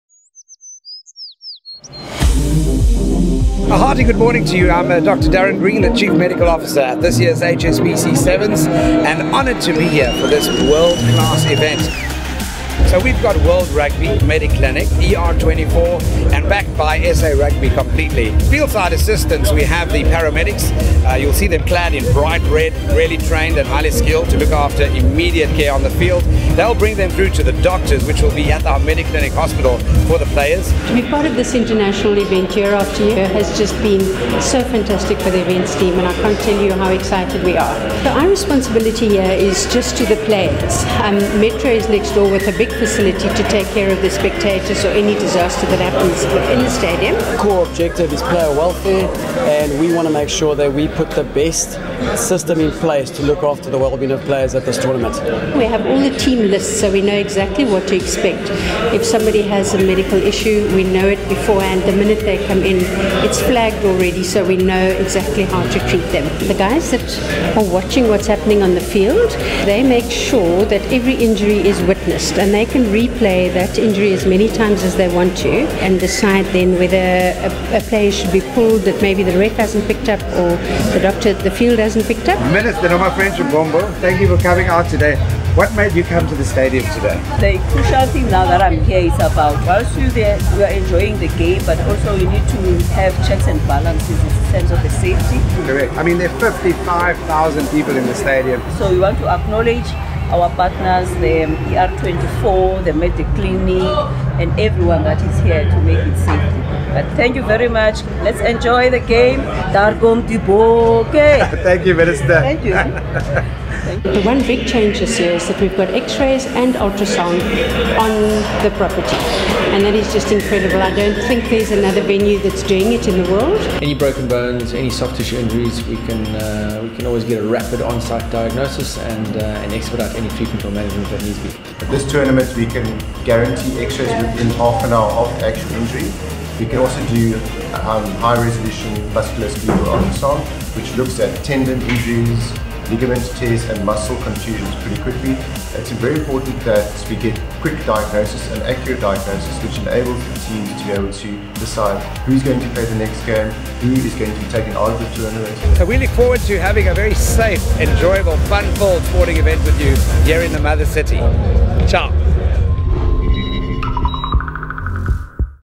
Behind the scenes at the HSCB Cape Town 7s
Rugby injuries can often cover a broad range of injuries, but this year Mediclinic and ER24 had the added benefit of onsite radiology and sonar to assist with swift and accurate diagnosis. Catch the inside scoop of our support of the popular HSBC Cape Town Sevens.